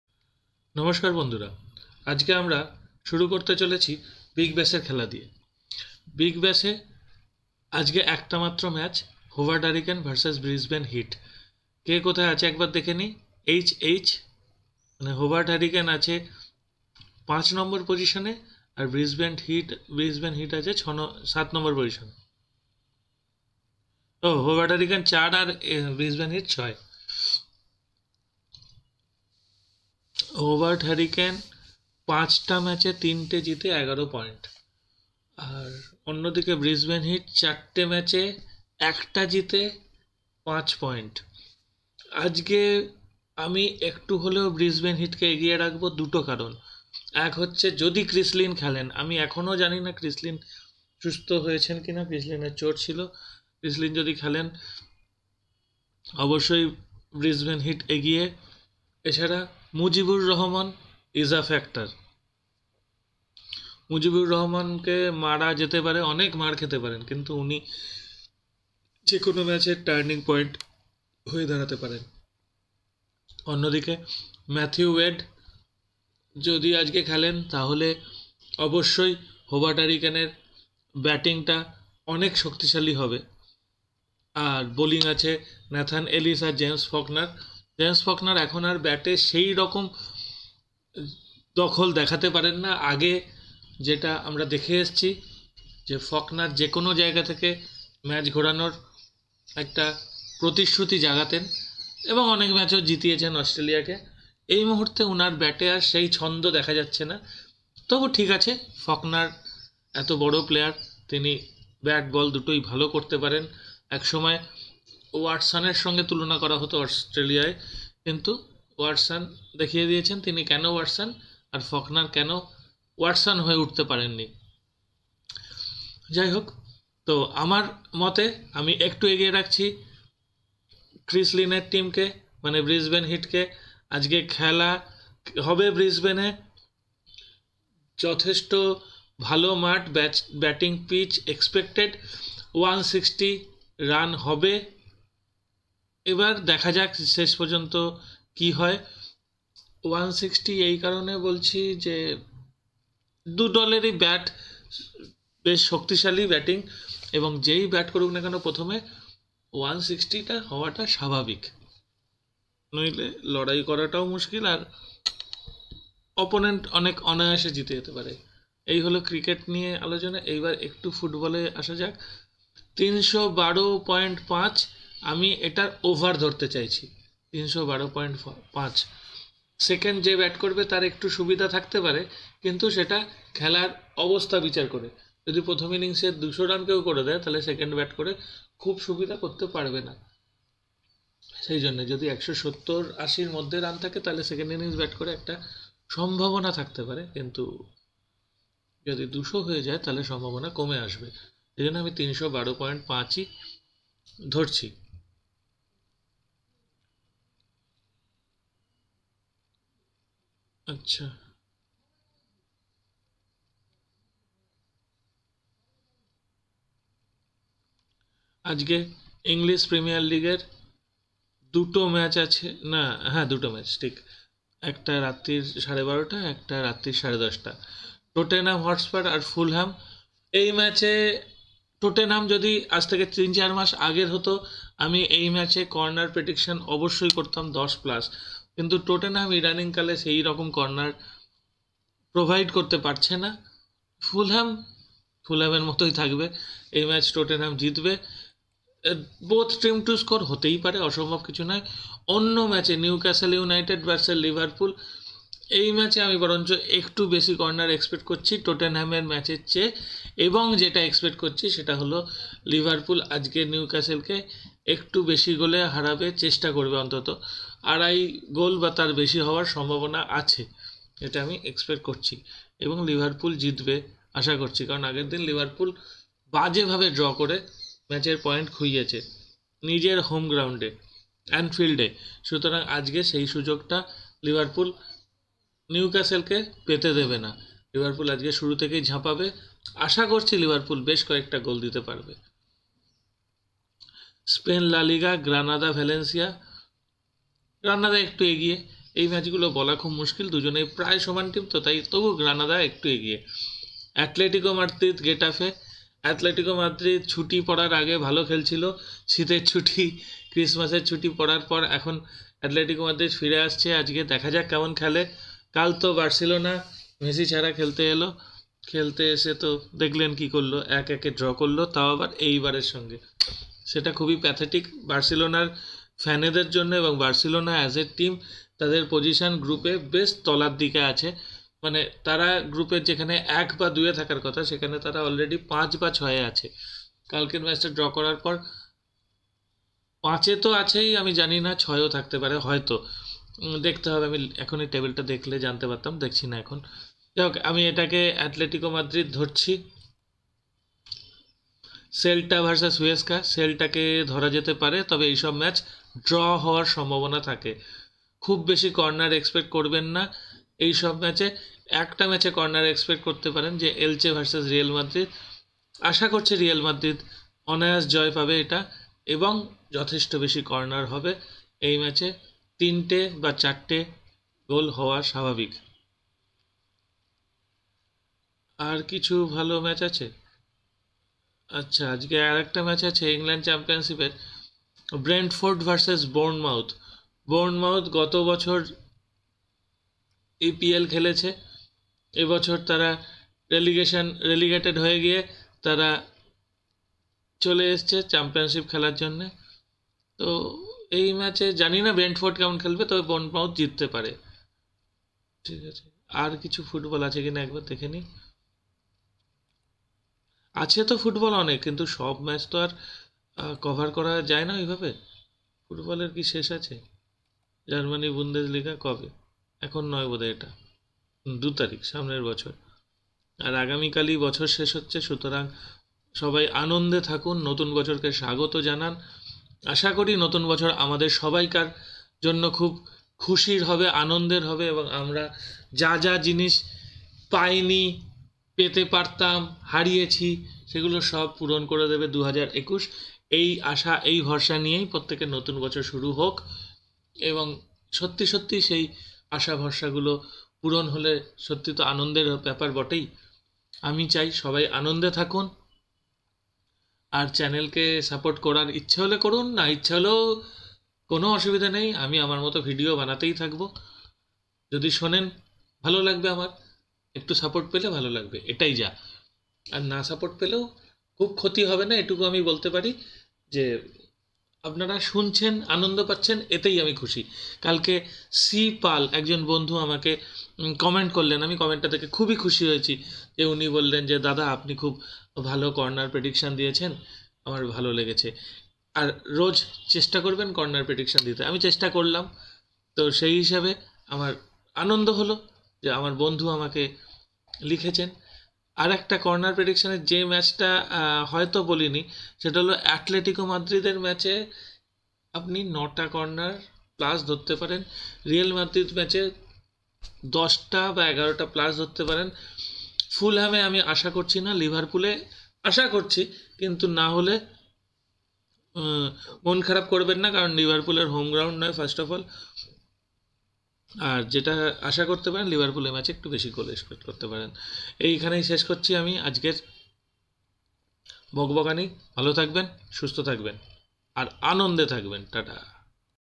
BBL:Hobart HurricanesV Brisbane Heat|Newcastle Utd v Liverpool |Elche vs Real Madrid
Big Bash League Match no 20th
Hobart HurricanesV Brisbane Heat

Tottenham Hotspur vs Fulham,
Newcastle United vs  Liverpool,

Spain La Liga
 Atletico Madrid vs Getafe,
 Elche  vs Real Madrid,

MY FAB INDIAN JERSEY
MY JERSEY